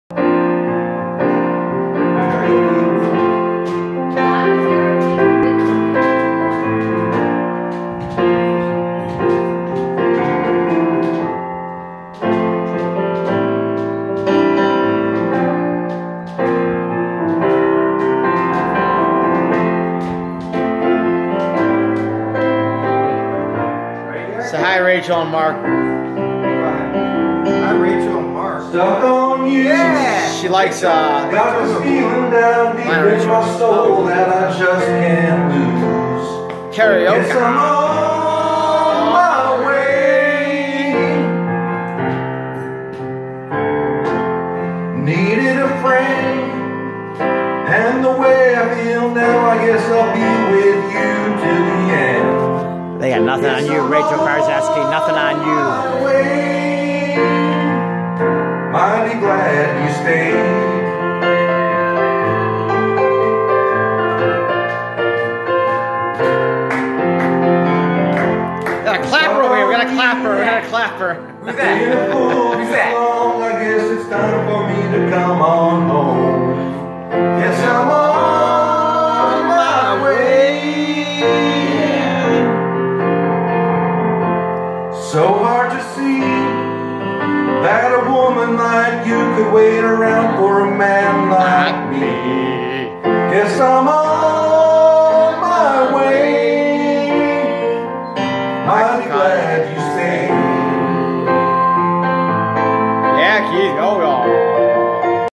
Mr . Keith Daniel Washo
Playing Lionel Ritchie sung by Derek Gennero and my Mom on her xxxx Birthday